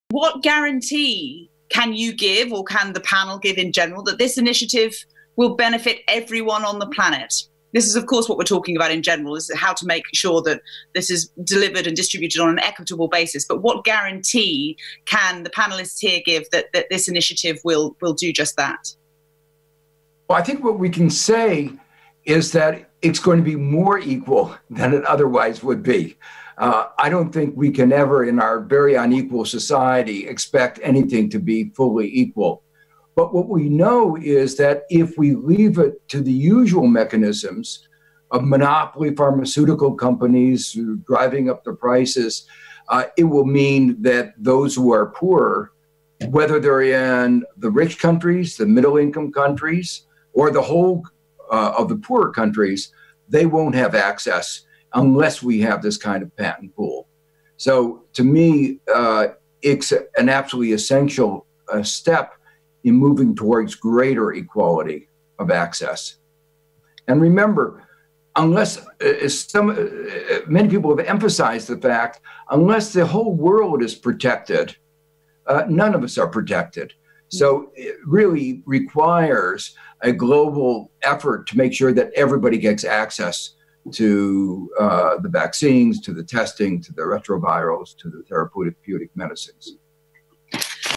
Professor Joseph Stiglitz at the C-TAP Solidarity Call to Action launch
On May 29, 2020, the World Health Organization (WHO) Director General Dr. Tedros Adhanom Ghebreyesus, the President of Costa Rica Carlos Alvarado Quesada, and 36 co-sponsoring member states launched the COVID-19 Technology Access Pool (C-TAP) with a Solidarity Call to Action. The WHO and the 37 co-sponsoring countries called upon key stakeholders and the global community “to voluntarily pool knowledge, intellectual property and data necessary for COVID-19.” The event was attended by several heads of states, government officials, academics, and leaders in the civil society community. This is a short clip of the interventions by Professor Joseph Stiglitz.

Statement of Knowledge Ecology International (KEI) on the launch of the C-TAP

Further information about KEI’s work relating to the coronavirus, including but not limited to the WHO C-TAP